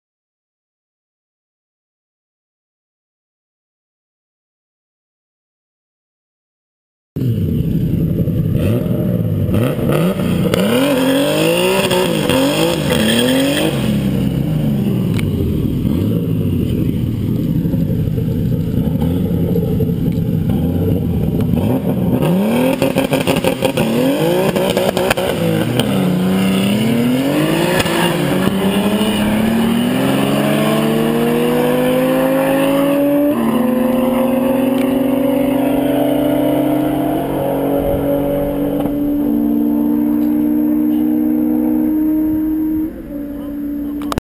Bmw e30 turbo vs. Suzuki gsx-r1000
Extremely even race from 0 up to about 250 km/h or 155 mph :)